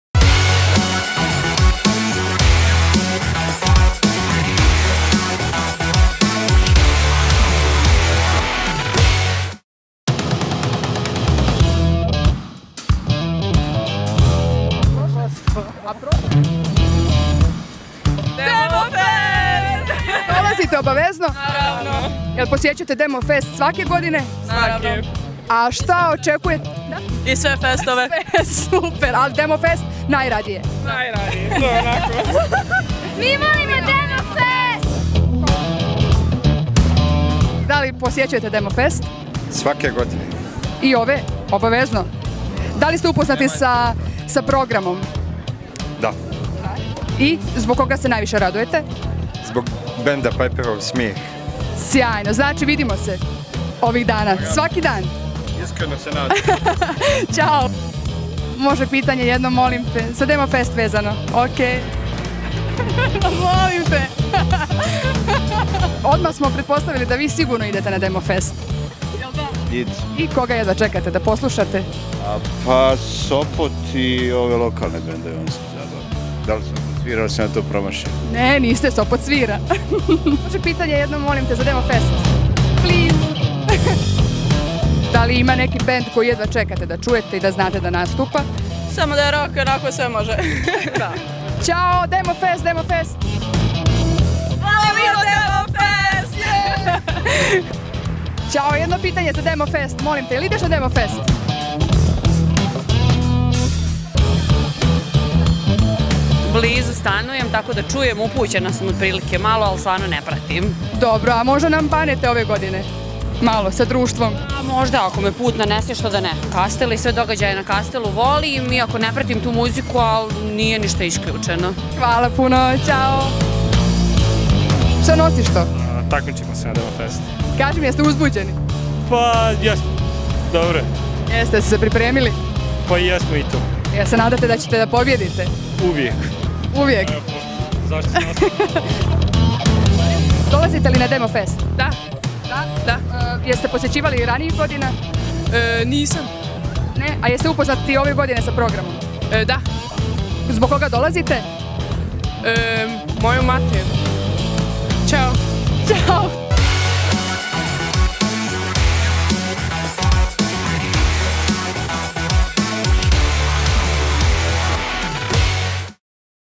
DF10 anketa - Gdje ide Banjaluka večeras?
Provjerili smo na ulicama Banjaluke gdje se ide večeras. Ako vas zanima, klik na play.